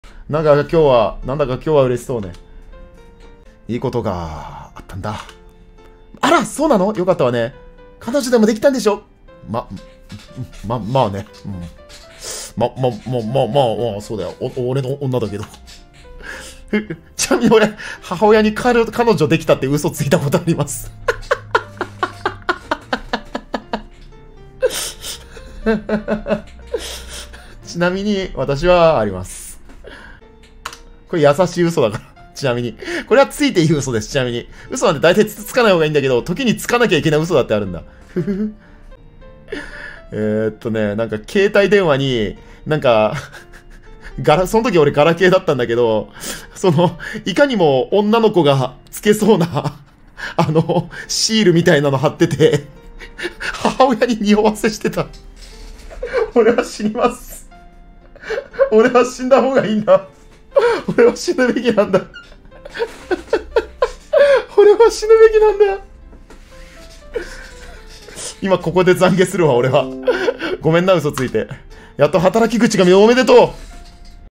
ゆゆうた、母についた嘘を懺悔【2023/03/15】
元配信
ゆゆうた押忍